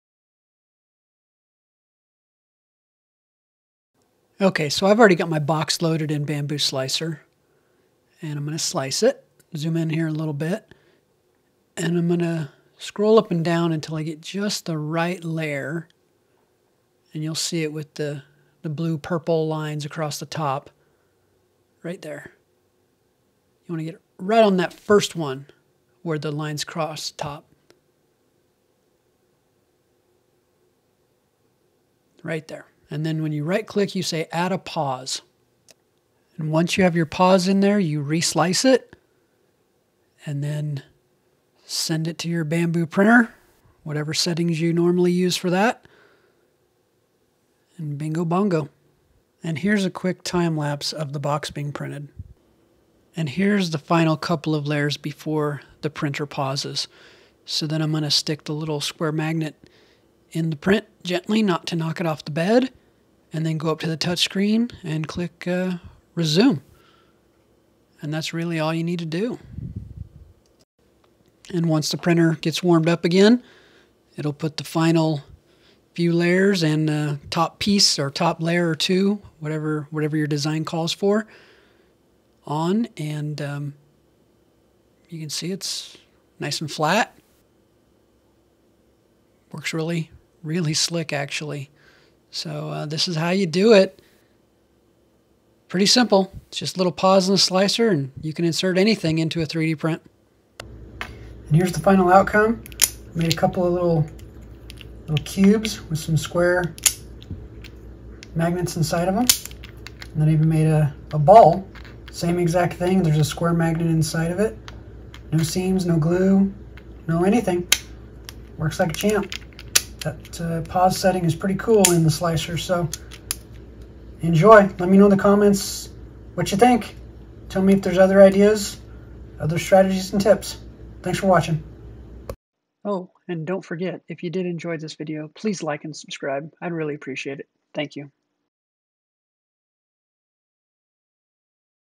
Insert Magnets (or anything) Into 3D Prints
Simple demonstration on how to use the pause features in Bambu slicer to be able to insert things into 3d prints, mid print. It's easier than you think.